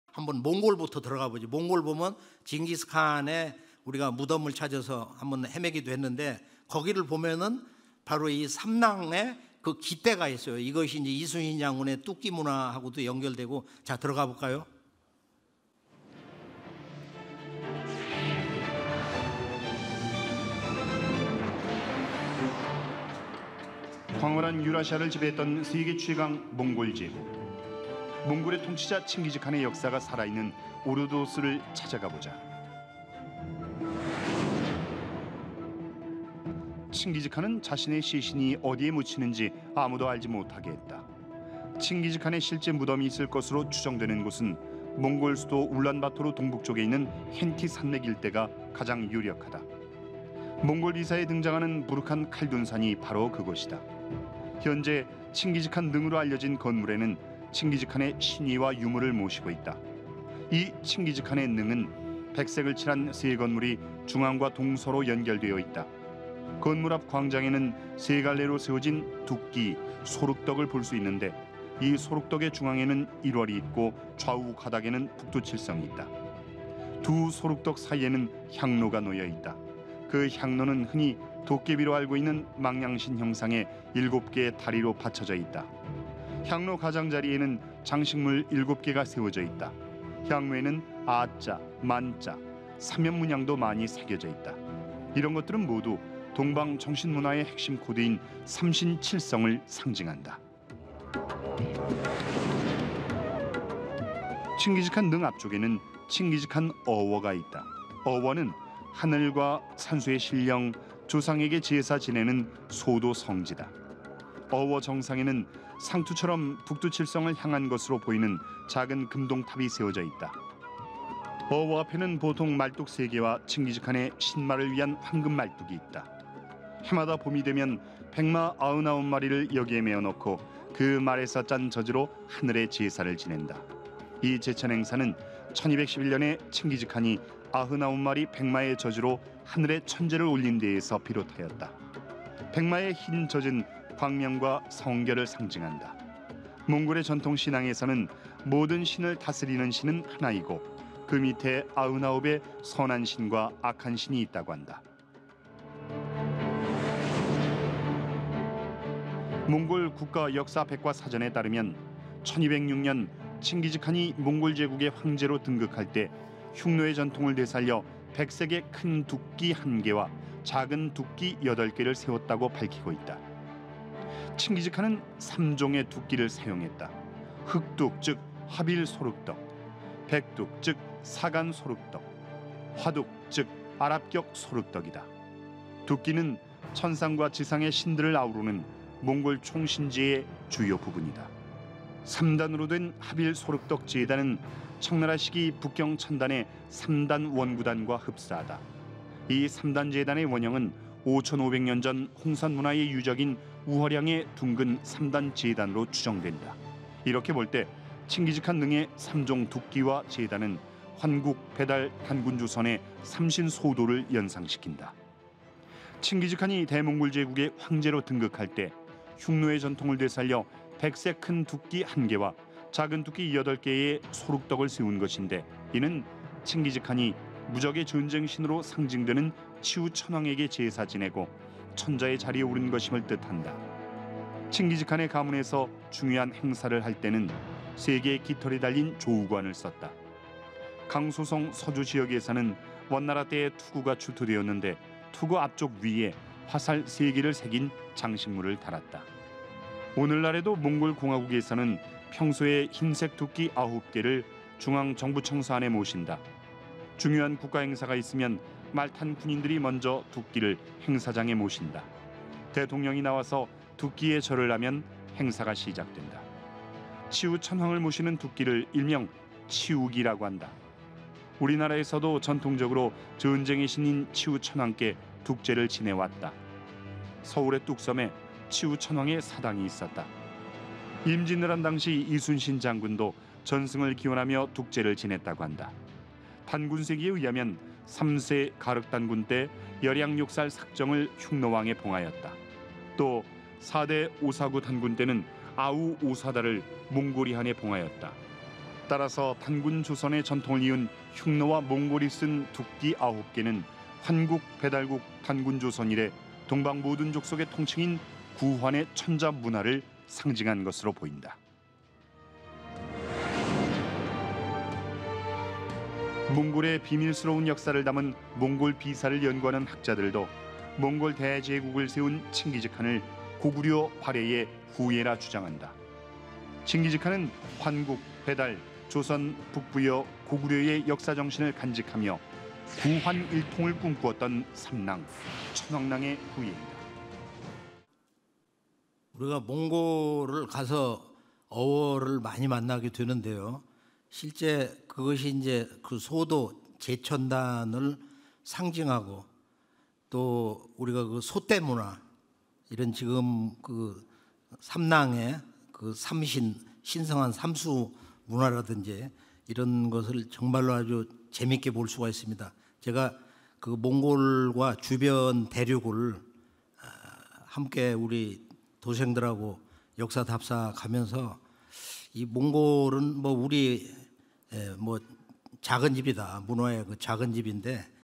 동방의 삼신三神 삼랑문화 몽골편ㅣ천부경과 홍익인간
[특별강연] 천부경과 홍익인간ㅣ안경전 대한사랑 상임고문ㅣ2021대한사랑국제학술대회